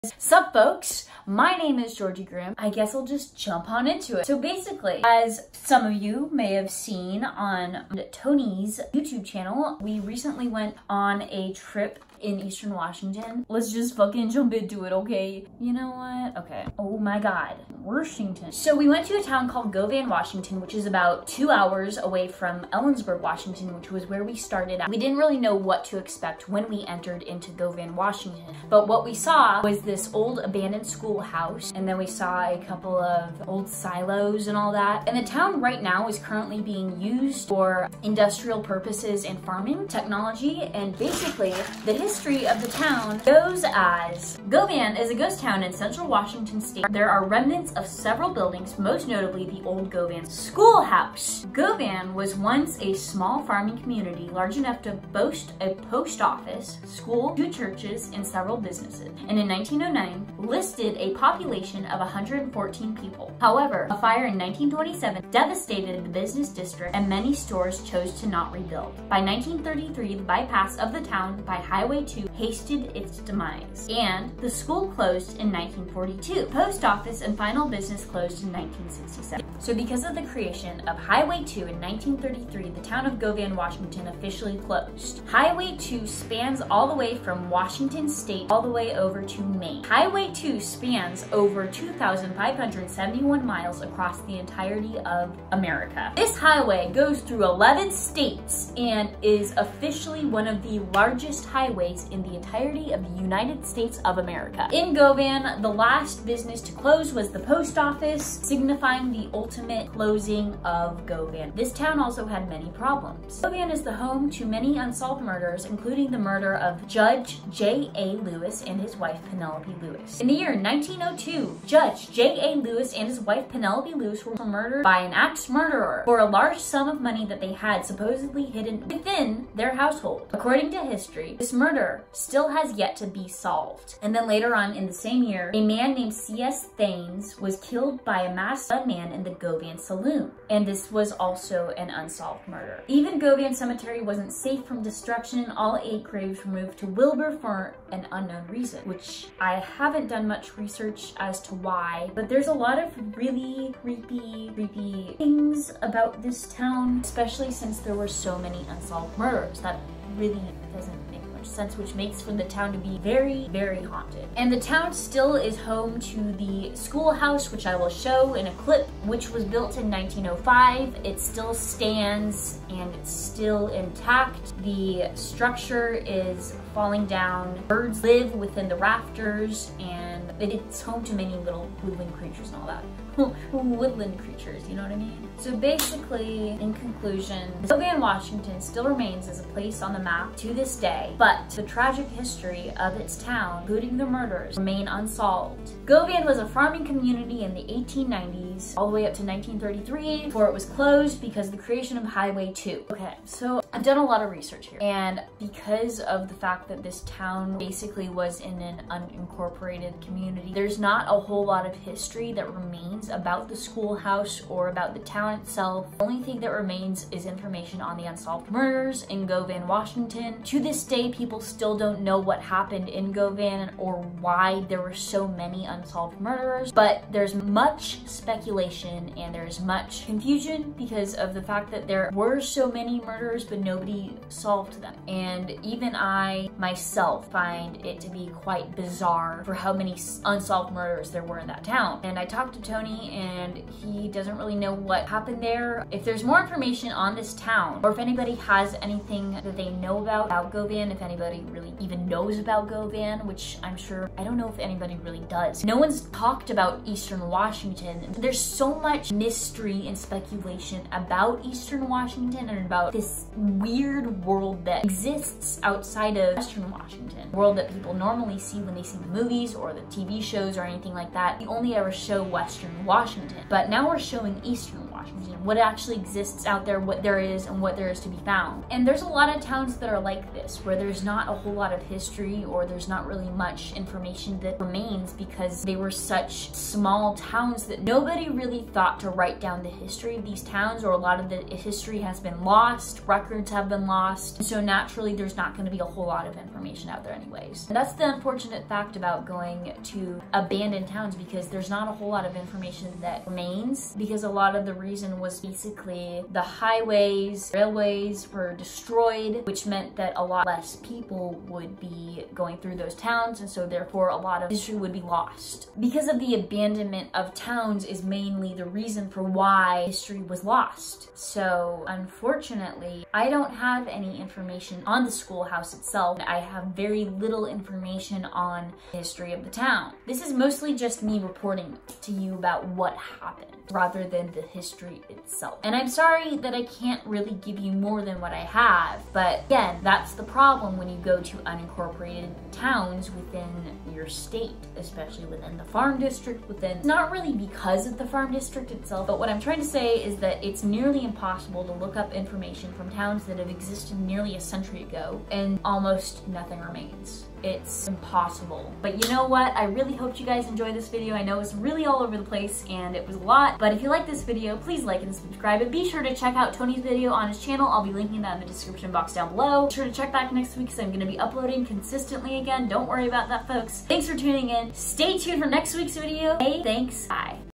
The History of Govan, WA | FORLORN
We explore Washington's ghost towns, but more importantly, I talk about the intriguing history of these ghost towns... Please enjoy!!!